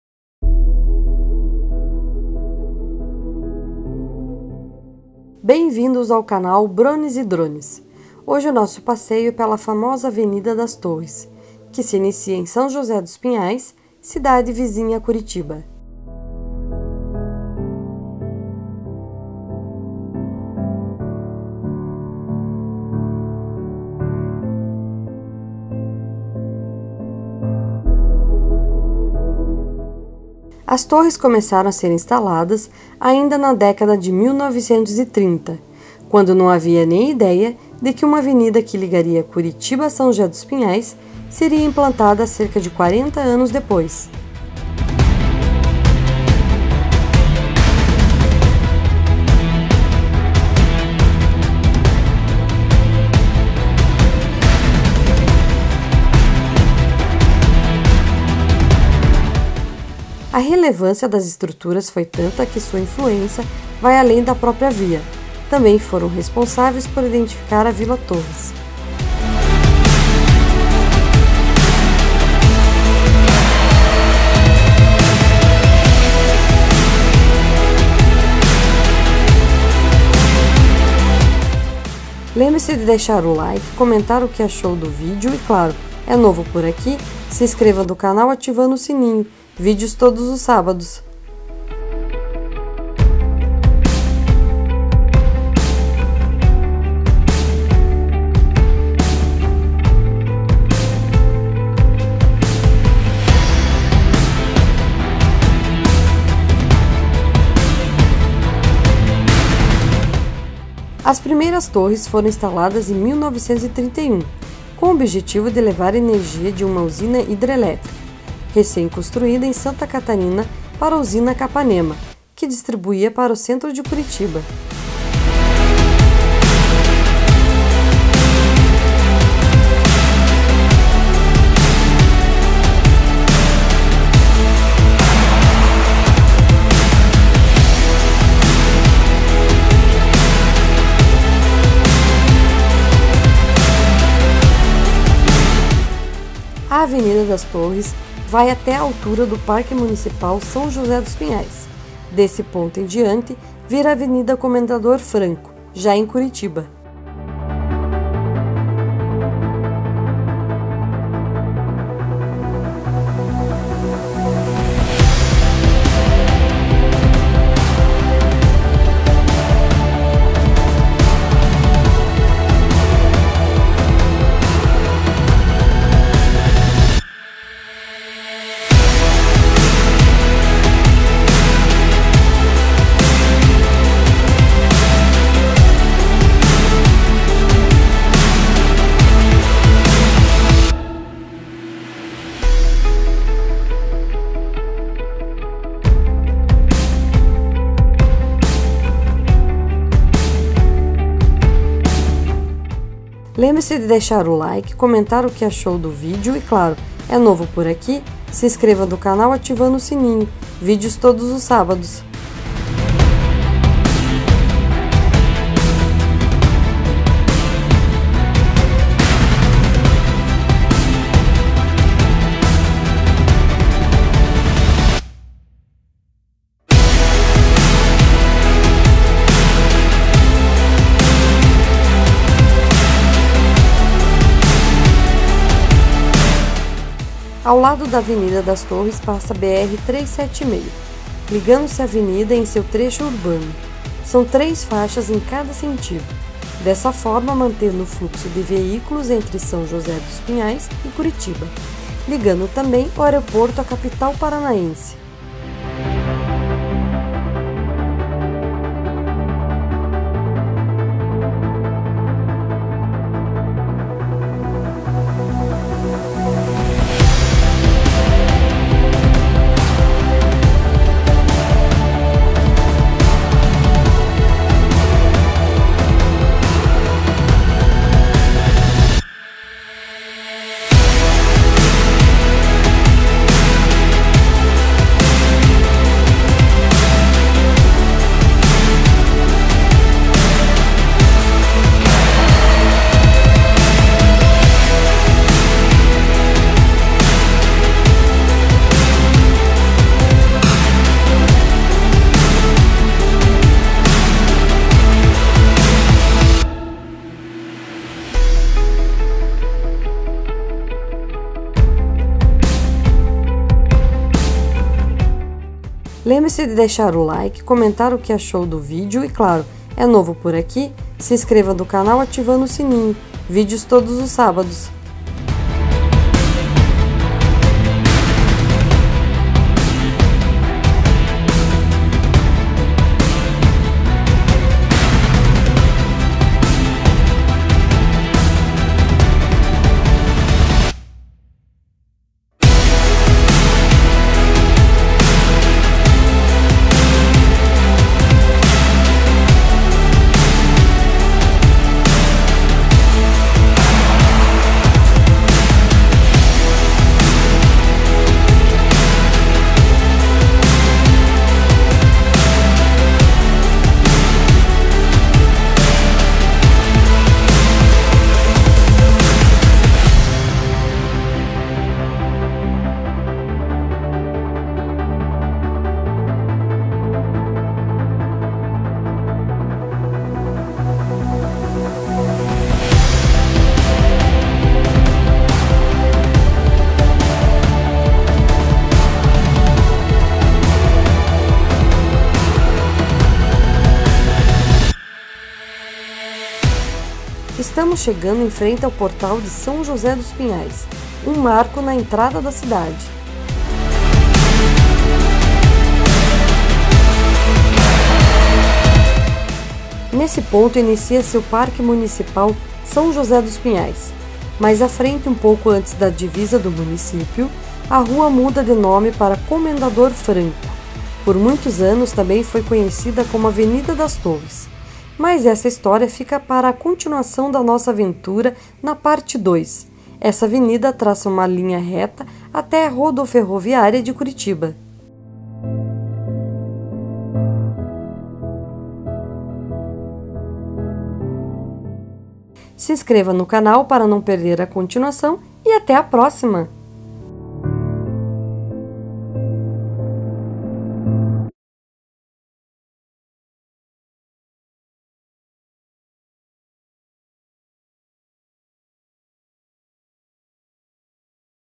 Avenida das Torres em São José dos Pinhais e Curitiba por drone - 1º Parte
Avenida das Torres em São José dos Pinhais e Curitiba no Parana, lindas imagens em 4k feita por drone receadas de informações e curiosidades.

1) Na década de 30
As primeiras torres foram instaladas em 1931, com o objetivo de levar energia de uma usina hidrelétrica recém-construída em Santa Catarina para a usina Capanema, que distribuia para o Centro de Curitiba. Na época, o terreno onde se encontram era apenas campo, e as terras tinham proprietários que permitiram a instalação das torres. Quem explica é o engenheiro Ivo Marcos Dranka Junior, gerente de engenharia e gestão de linhas de transmissão da Companhia Paranaense de Energia (Copel). “A Copel, que então se chamava Companhia de Força e Luz, decidiu instalar as torres ali para traçar uma linha reta entre dois pontos específicos”, conta Junior. Segundo o engenheiro, já naquela época havia planos de construir uma rede subterrânea, o que só vai se concretizar este ano.

2) A avenida surgiu em função das torres
Segundo o engenheiro Ivo Dranka Junior, da Copel, a instalação das torres implicou a inutilização do espaço logo ao lado das estruturas. Trata-se de uma faixa de segurança que facilita a manutenção da rede elétrica. “Como não é permitido construir casas ou prédios no terreno dos dois lados da linha elétrica e o caminho é muito bom por formar uma linha reta, a prefeitura costuma aproveitar esses pedaços para tráfego de veículos”, avalia o engenheiro. A Avenida Comendador Franco foi idealizada e viabilizada na década de 70, junto da inclusão de novas torres na linha elétrica.

3) Que fim levarão as torres?
A Copel explica que as torres que estão sendo removidas da avenida não serão reutilizadas em outro local. Isso porque os materiais são antigos e não comportam as voltagens usadas atualmente. Por exemplo, a capacidade energética usada até então era de 138 mil volts. Com a nova instalação, o número deve passar para 230 mil volts. De acordo com a empresa, por enquanto as torres serão desmanchadas e armazenadas no galpão da companhia.

4) Av. das Torres só começa em São José dos Pinhais
Muitas pessoas não sabem que a maior parte da via popularmente conhecida como Avenida das Torres se chama, oficialmente, Avenida Comendador Franco. Mas o nome só é válido até a altura do Parque Náutico, um pouco antes do início do município de São José dos Pinhais. Na cidade vizinha à Curitiba, sim, a via passa a se chamar Avenida das Torres.

5) Correios reconhecem nome não-oficial
Como existem pessoas que confundem o nome da avenida, os Correios acabam aceitando fazer a entrega de correspondências que descrevem a parte curitibana como Avenida das Torres. Segundo a assessoria de imprensa da empresa, não é incomum que isso aconteça. O único requisito primordial para que a entrega aconteça é que o CEP esteja correto."

Me Sigam no facebook, instagram e twitter para mais novidades sobre novos lançamento.

Conheça os nossos canais parceiros